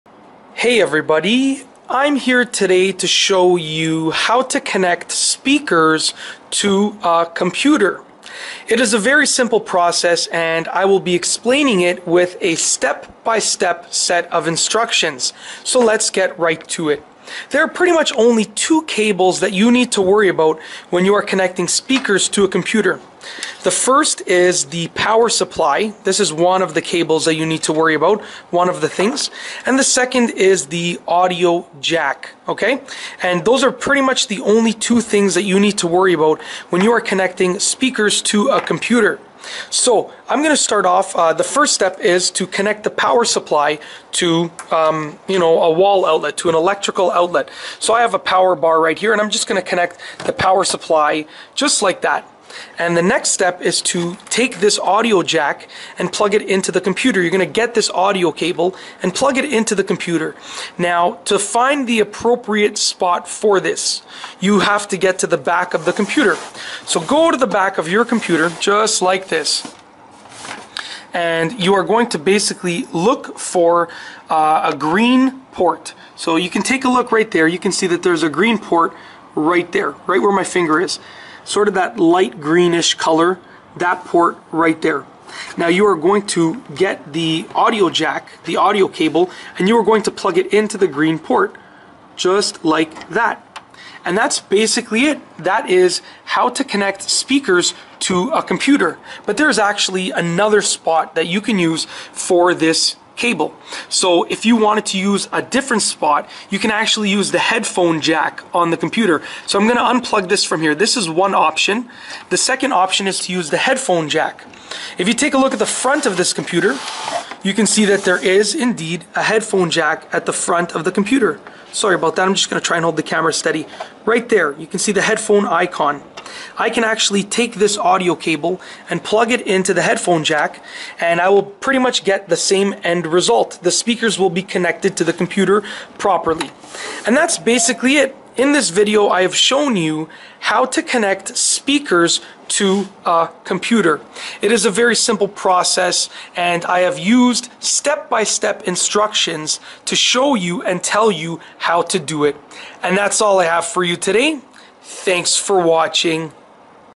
How To Connect Speakers To A Computer (Tutorial)
In this video, I show you how to connect speakers to a computer. It is a tutorial that is geared towards showing you one of the basic computer connections that you will likely have to make when connecting the components of a desktop computer. I outline 2 specific ports that you can use to connect speakers to a desktop computer.